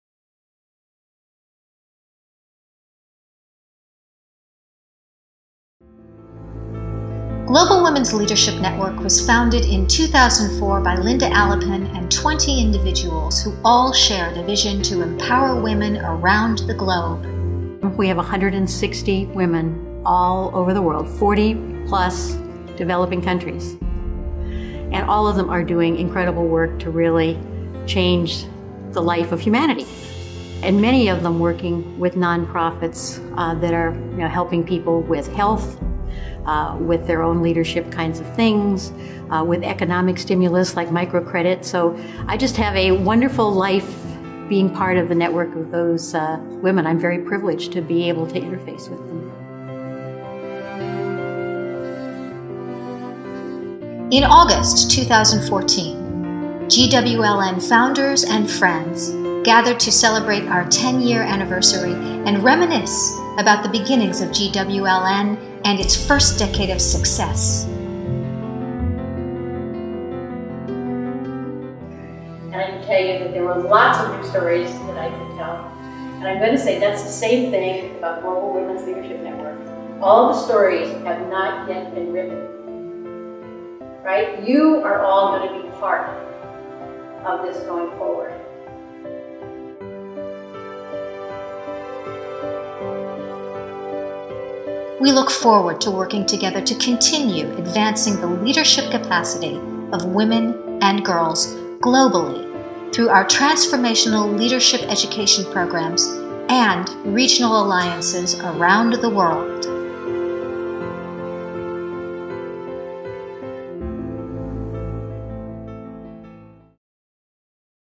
GWLN Founders Celebrate a Decade of Empowering Women Around the Globe
GWLN was established in 2004 by Linda Alepin and 20 co-founders who all shared a vision to empower women around the globe. They believed passionately in a world where women can step out of survival and dependence into their inherent power, enabling the transformation of world conditions - woman by woman. Today, the co-founders are continuing to connect and collaborate to realize their vision of a better world through the leadership development of women and girls globally.